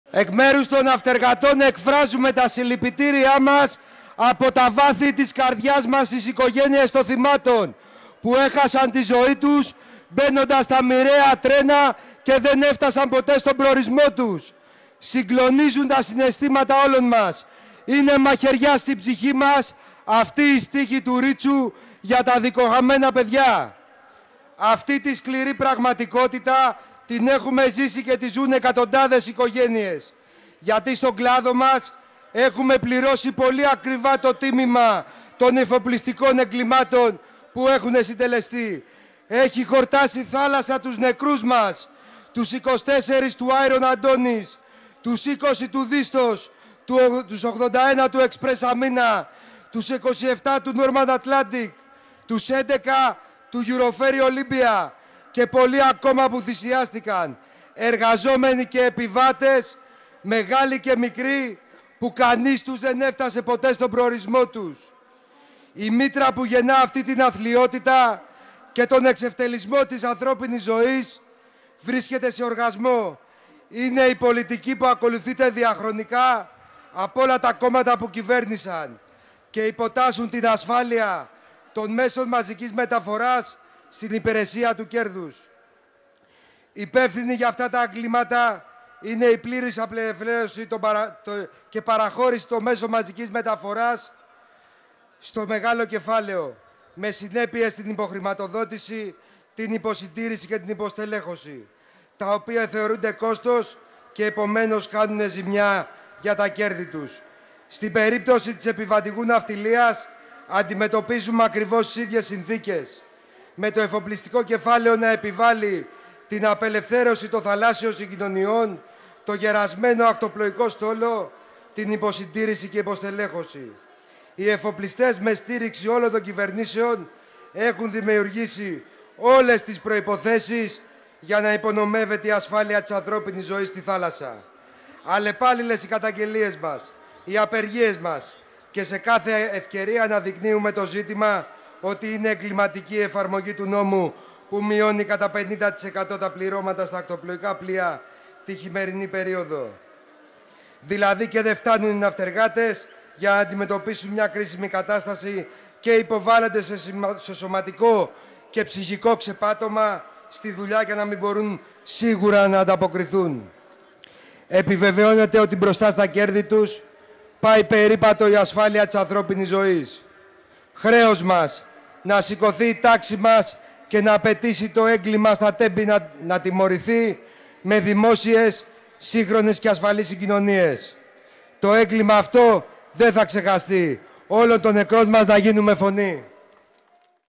Ομιλία του Αγγ. Γαλανόπουλου στην απεργιακή συγκέντρωση της Αθήνας
Ένα απέραντο ποτάμι οργής και αγώνα σχημάτισαν στην Αθήνα δεκάδες χιλιάδες διαδηλωτές.

Καμία συγκάλυψη των πραγματικών ενόχων για το έγκλημα στα Τέμπη απαίτησαν και ανανέωσαν το ραντεβού για το νέο μεγάλο συλλαλητήριο την Κυριακή 12 Μαρτίου στις 12 το μεσημέρι στο Σύνταγμα.